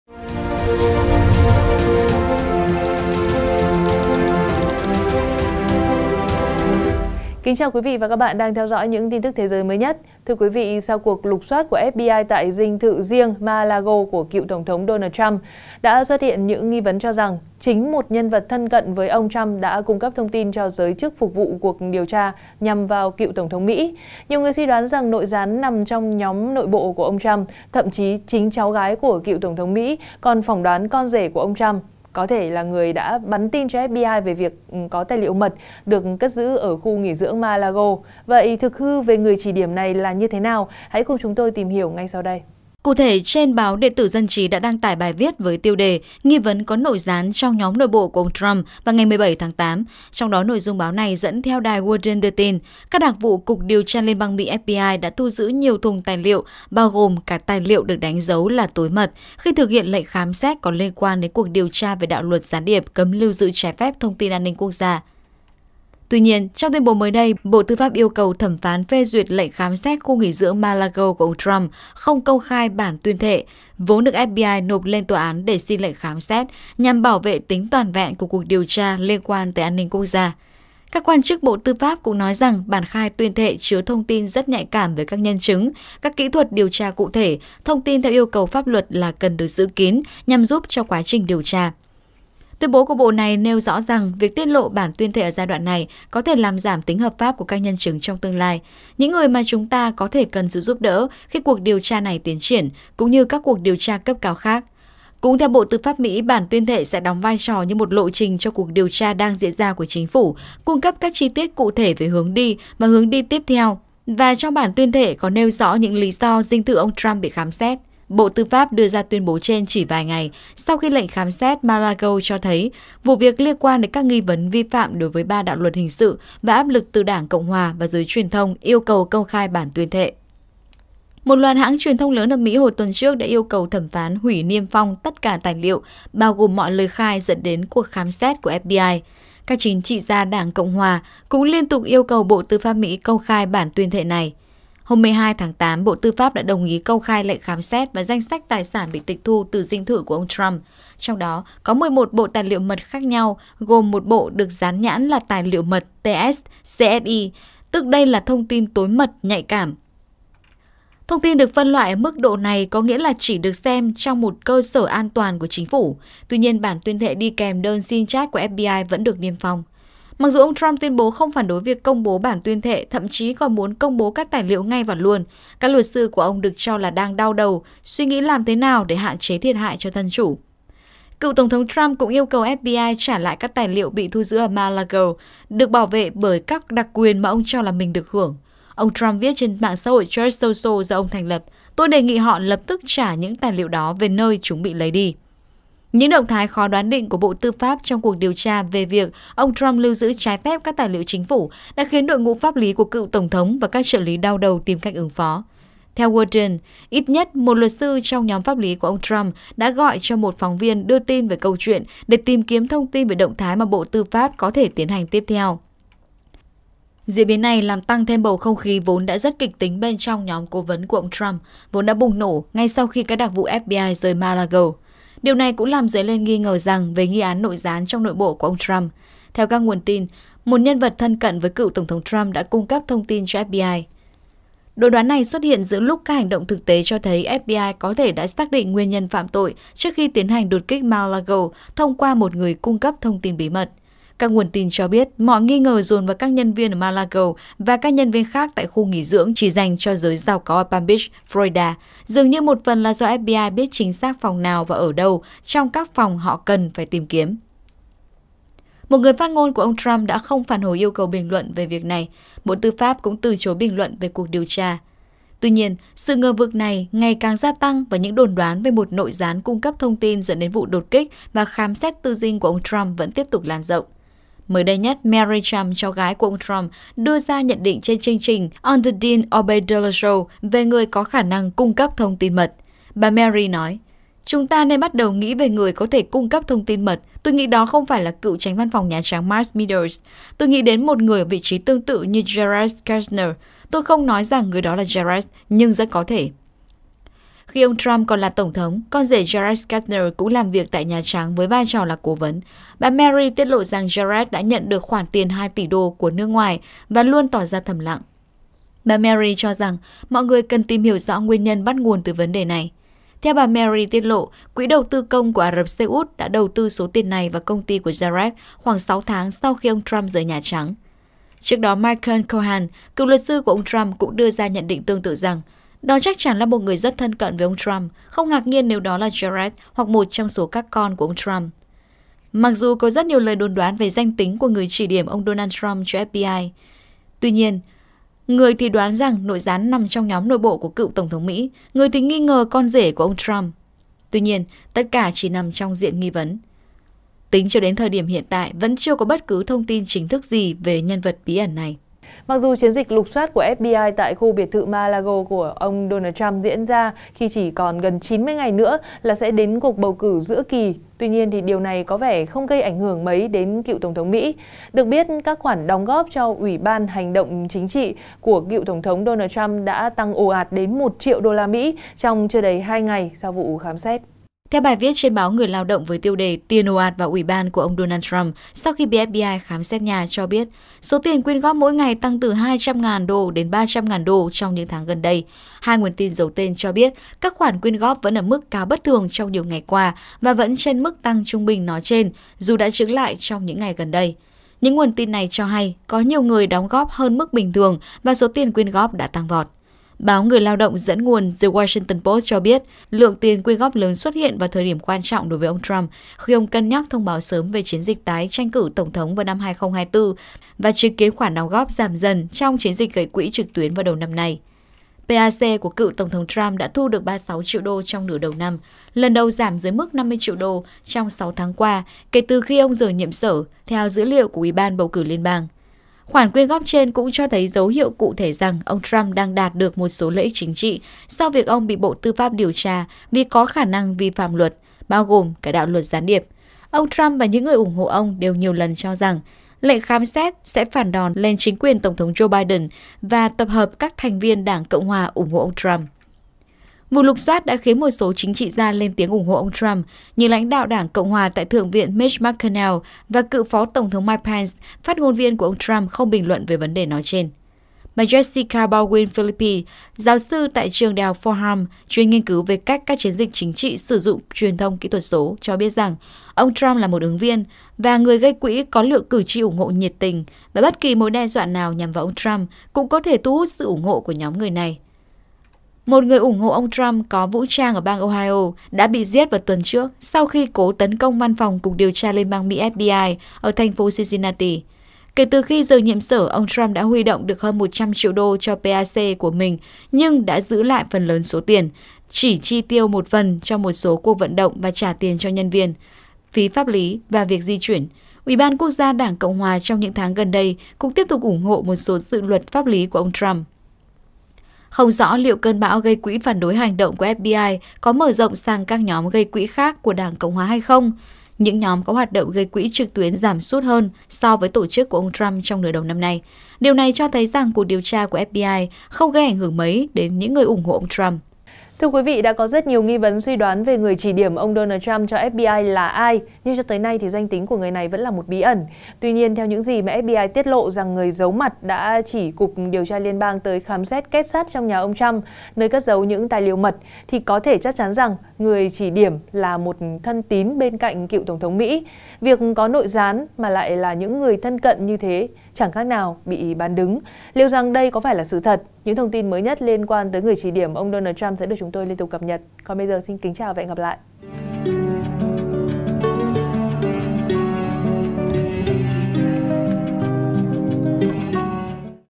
Nghi Vấn Tìm Thấy Người Chỉ Điểm Ông Trump Cho FBI, Nội Gián Là Con Rể Hay Trong Nhóm Nội Bộ? | SKĐS
SKĐS | Sau cuộc lục soát của FBI tại dinh thự riêng Mar-a-Lago của cựu tổng thống Mỹ Donald Trump, đã xuất hiện những nghi vấn cho rằng chính một nhân vật thân cận với ông Trump đã cung cấp thông tin cho giới chức phục vụ cuộc điều tra nhằm vào cựu tổng thống Mỹ. Nhiều người suy đoán rằng nội gián nằm trong nhóm nội bộ của ông Trump, thậm chí chính cháu gái của cựu tổng thống Mỹ còn phỏng đoán con rể của ông Trump, có thể là người đã “bắn tin” cho FBI về việc có tài liệu mật được cất giữ ở khu nghỉ dưỡng Mar-a-Lago. Vậy thực hư về người chỉ điểm này là như thế nào? Hãy cùng chúng tôi tìm hiểu ngay sau đây.

Mời quý vị và các bạn xem thêm: Ngấm Đòn Cắt Khí Đốt Của Nga, Châu Âu Vạch “Lằn Ranh Đỏ” Trước Nguy Cơ Mùa Đông Lạnh Giá